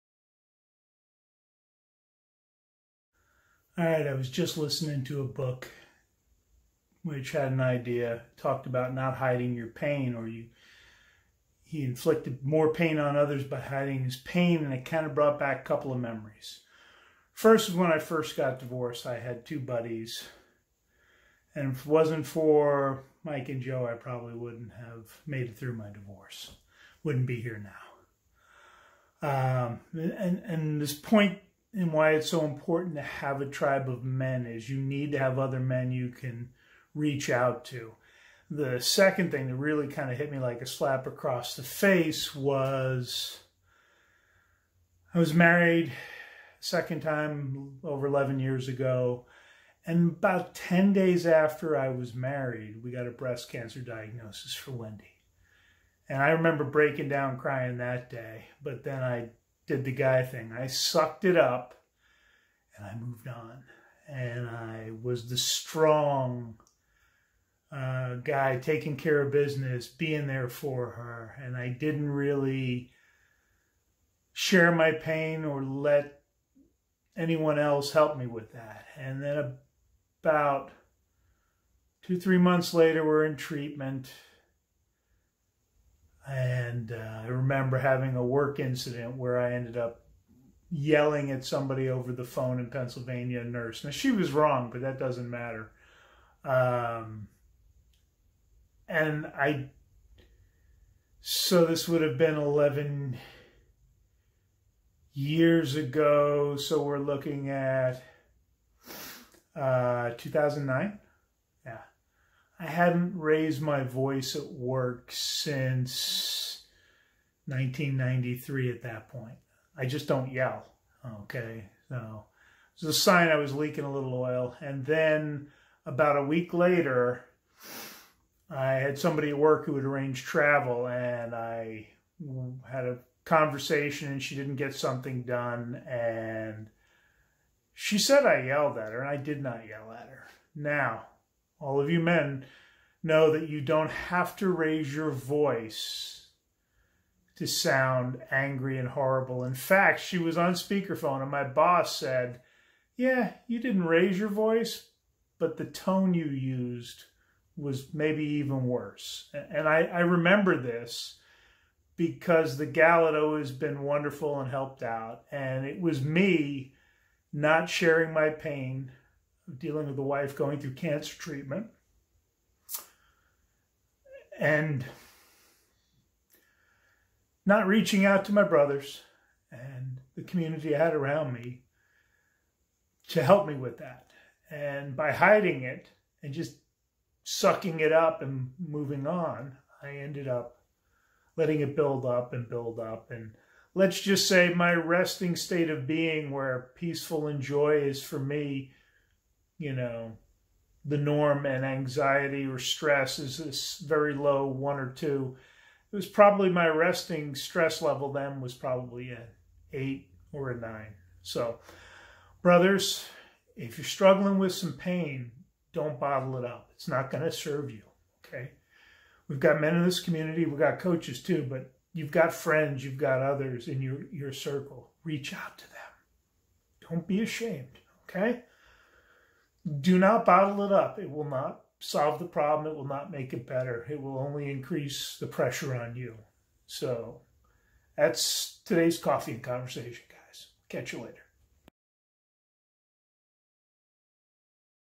Don't hide your pain!
This week's coffee & conversation is about a common mistake we make as men.